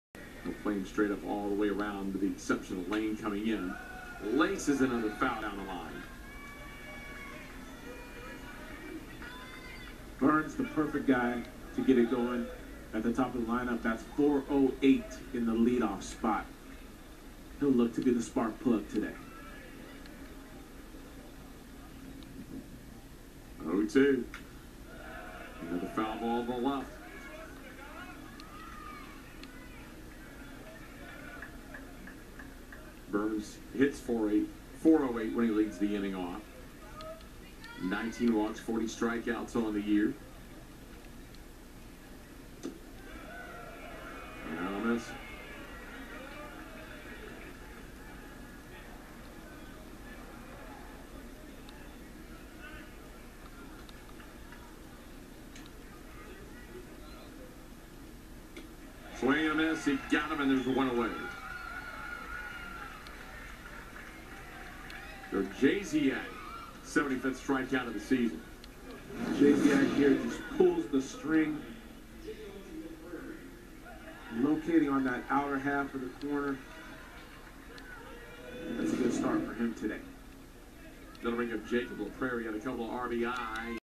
USF P Jack Jasiak vs. Tulane's Collin Burns in American Conference tourney
USF(South Florida) pitcher Jack Jasiak had quite a game vs. Tulane in the American Conference tournament.  He threw a shutout.   Here he faces Tulane's Colllin Burns, 5/27/21 at Montgomery, Alabama's Riverwalk Stadium.